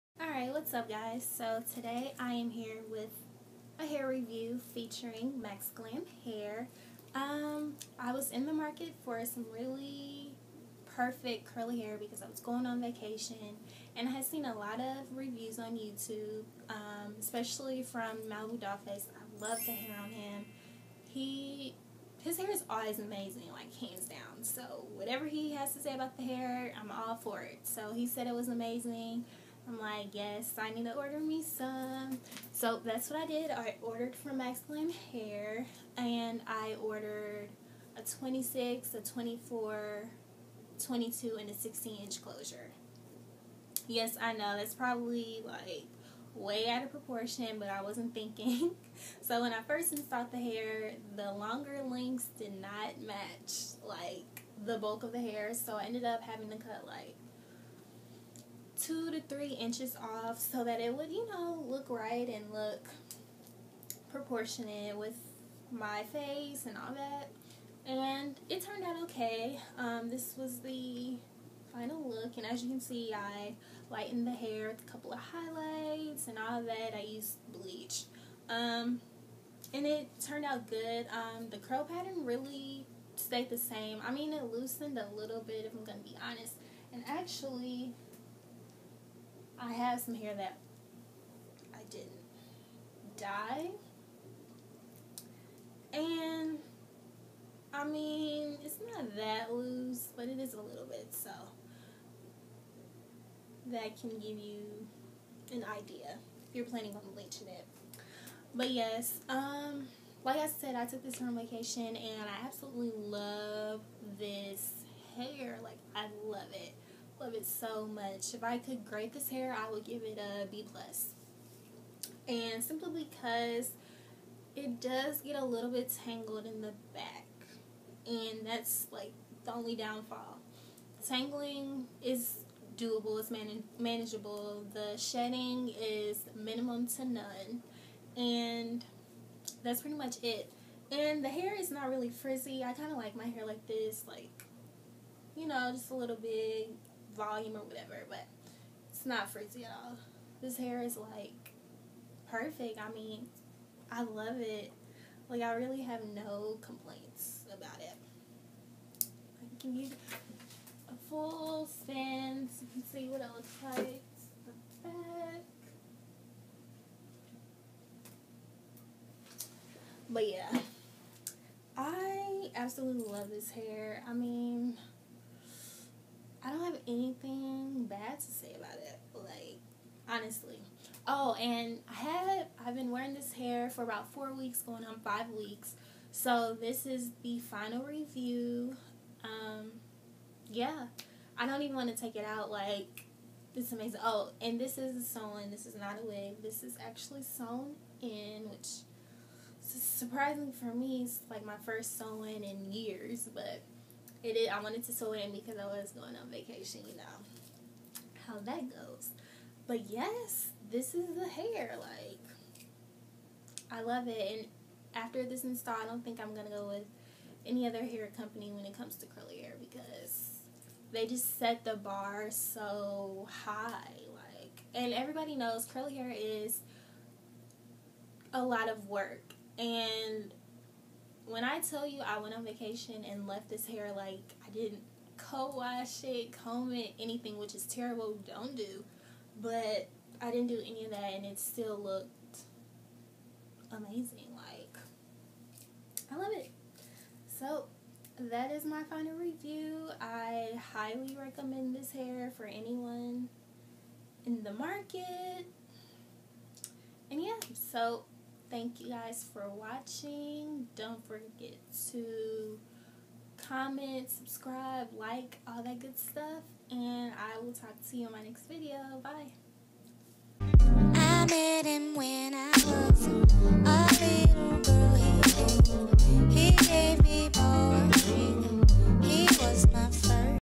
Customer Review - Maxglam Hair l Coloring & Highlight Brazilian Deep Wave Curly Hair
Hey glams, this is a review from our beautiful customer - Channel: Samone
Hair Specs: 22 24 26 with 16in closure
Add the lengths you need and the closure to cart together to place an order! Best deals for you now!